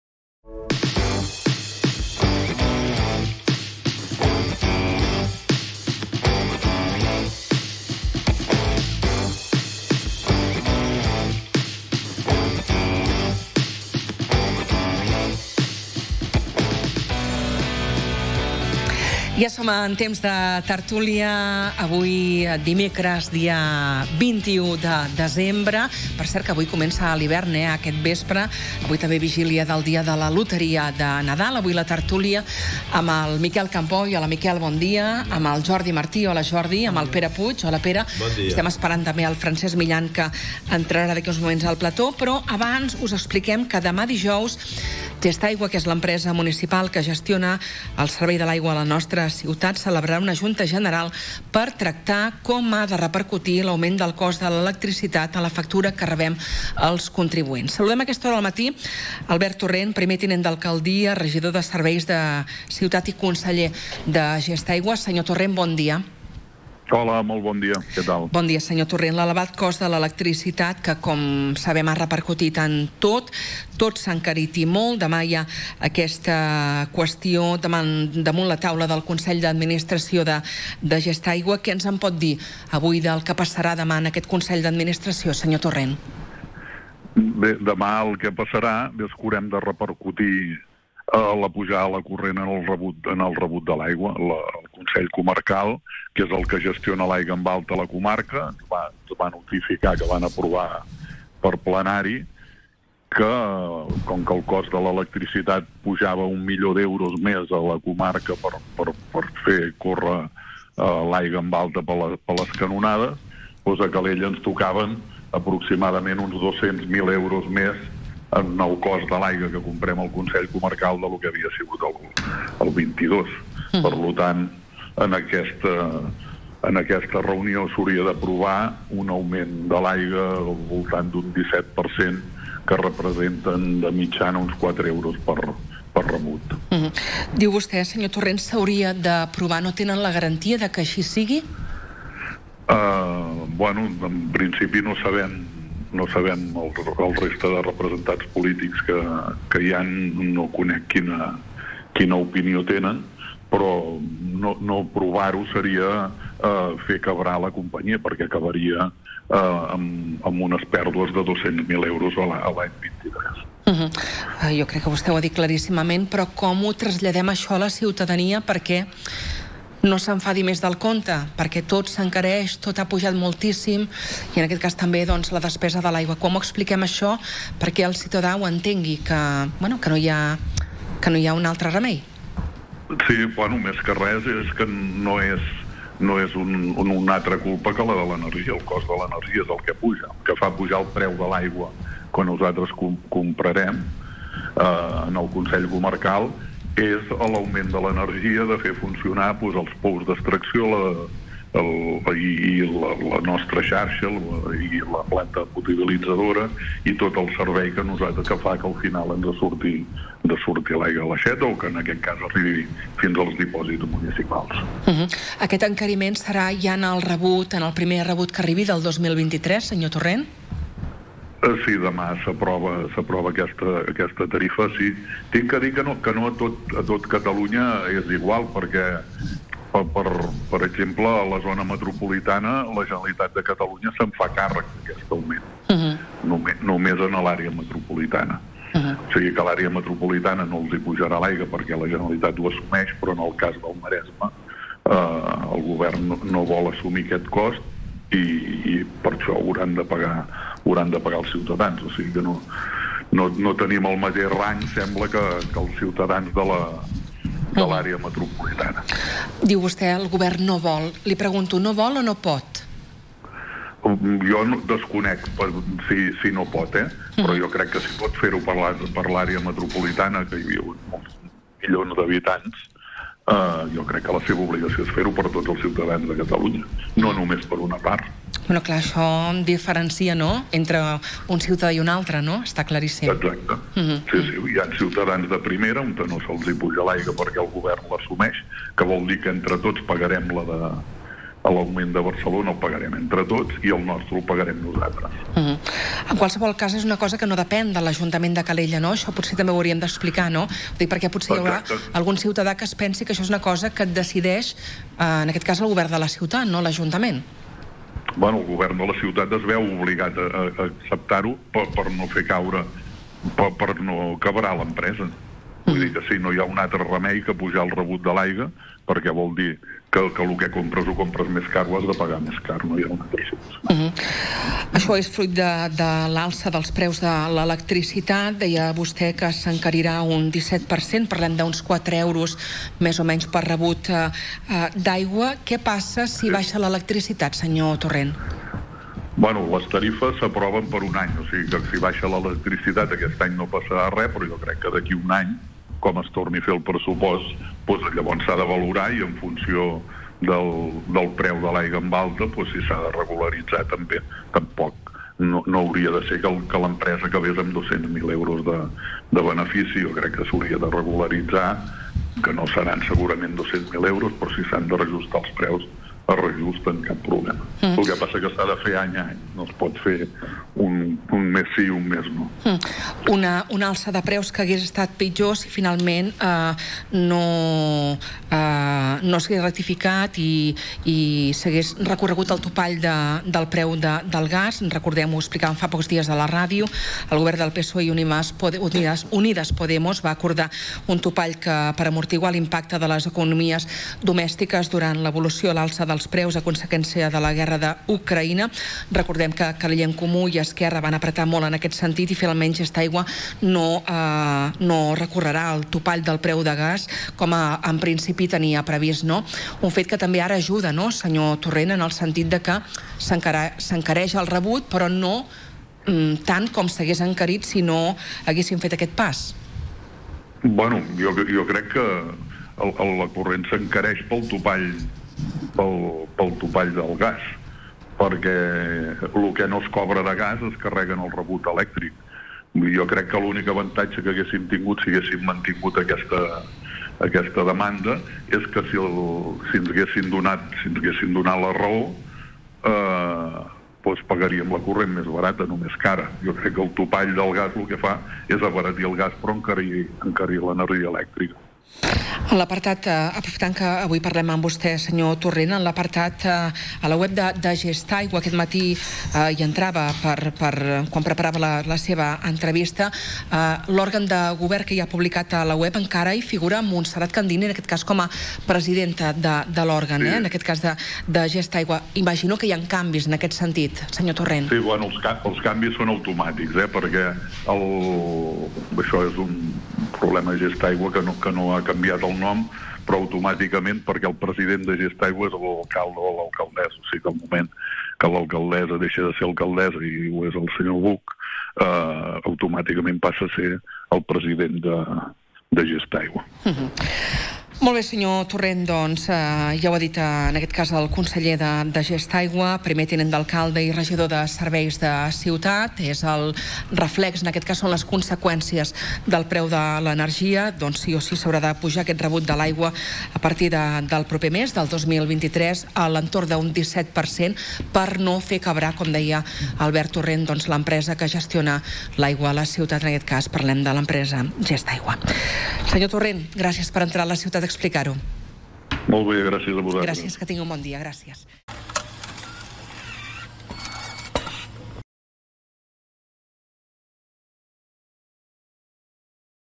Entrevista Albert Torrent
Gestaigua, l’empresa municipal que gestiona l’aigua a la nostra ciutat, celebrarà un Consell d’Administració per tractar com ha de repercutir l’augment del cost de l’electricitat en la factura que rebem els contribuents. Parlem amb Albert Torrent, primer tinent d’Alcaldia, regidor de Serveis de Ciutat i Conseller Delegat de Gestaigua.